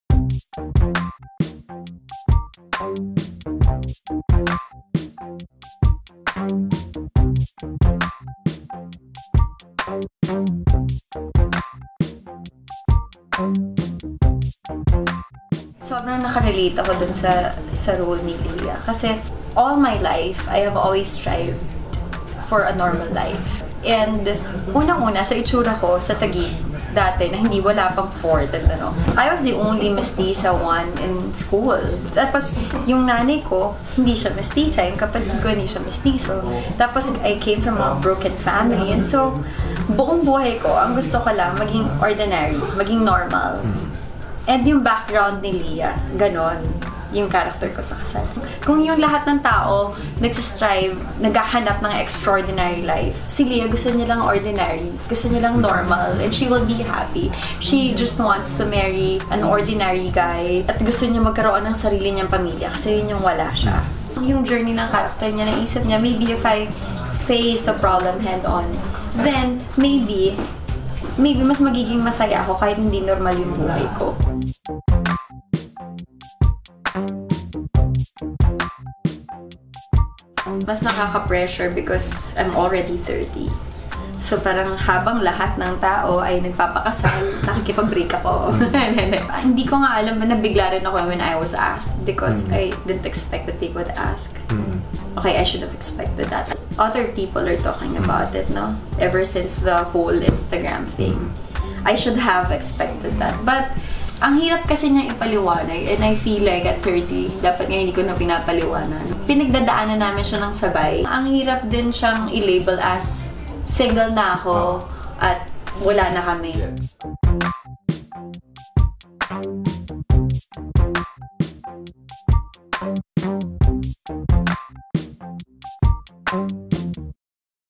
Bea Alonzo starts again
"I don't know where it's going to go. If it's going to be a final breakup or... you know? I don't really know. Ang hirap rin niya i-label na, 'Single na ako,' at 'Wala na kami,' habang nag uusap pa kami at tinatrabaho pa, baka meron pang..." She let's that statement linger.

Bea Alonzo opens up to Supreme about relationships, turning 30, and how she could relate to her character Lia in the new movie 'Kasal,' alongside Paulo Avelino and Derek Ramsay.

Video by Chel Sudoy
Photo by Regine David
Creative Direction by David Milan
Styled by Stylelist Inc.
Makeup by Ting Duque
Hair by Ayu Manaoag